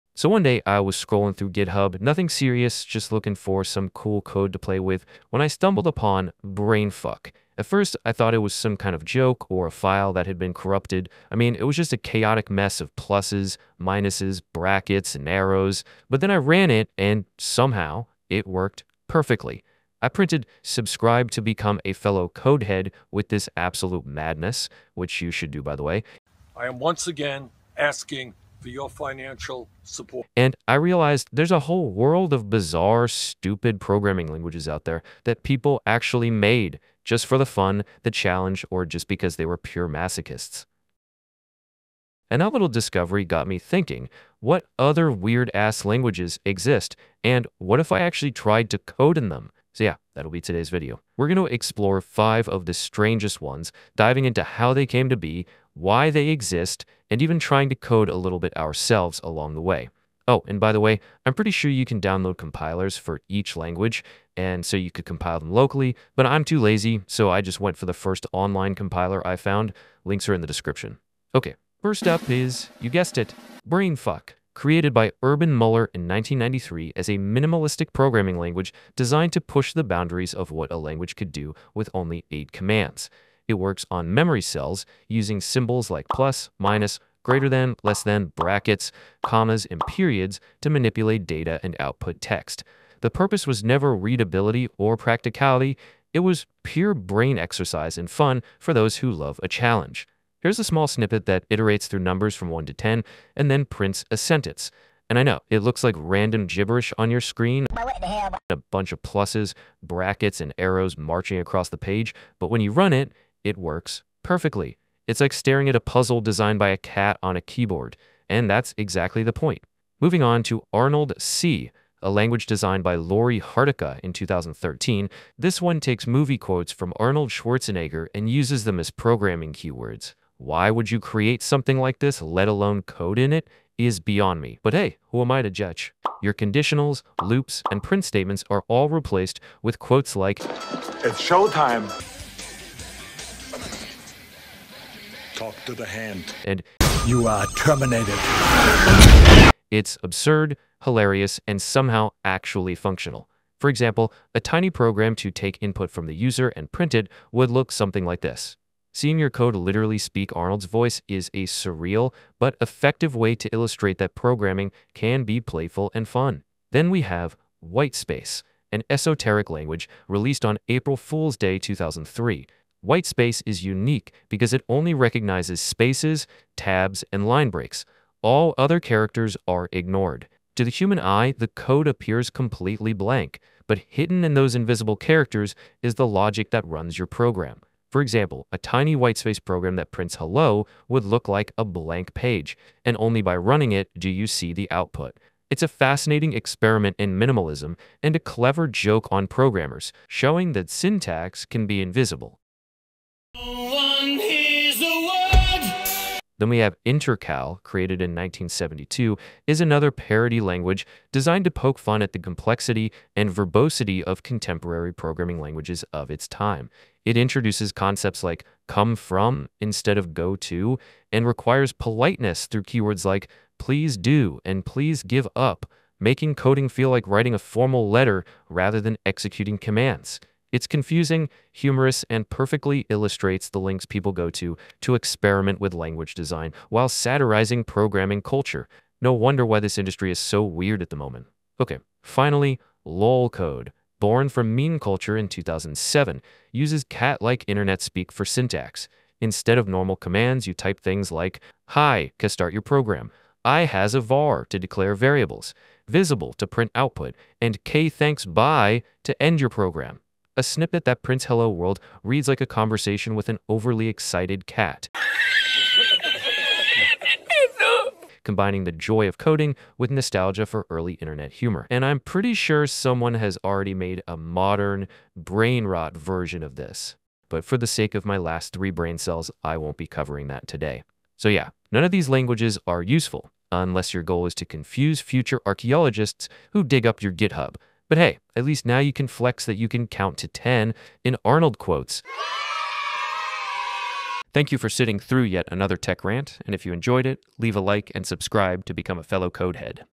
I Tried The WEIRDEST Programing Languages
👉I made a Discord server for all of you code heads to join

I accidentally stumbled into the weirdest programming languages on GitHub… and let me tell you, they are absolutely bonkers! In this video, I try coding in Brainfuck, ArnoldC, Whitespace, INTERCAL, and LOLCODE—yes, some of these languages are literally unreadable, hilarious, and completely useless… but insanely fun. Watch me struggle, laugh, and somehow make code work in the most absurd ways possible.

From Brainfuck loops that look like cat chaos, to Arnold Schwarzenegger quotes as code, invisible Whitespace commands, polite-yet-confusing INTERCAL, and meme-inspired LOLCODE, this is the ultimate “what the heck is programming?” experience. Perfect for developers, coders, or anyone who loves weird tech, esoteric languages, and programming challenges that make you question reality.

Try these languages yourself (links to online compilers in the description) and see if you can survive Brainfuck, whisper to ArnoldC, or chat with LOLCODE cats. If you think coding is serious… think again.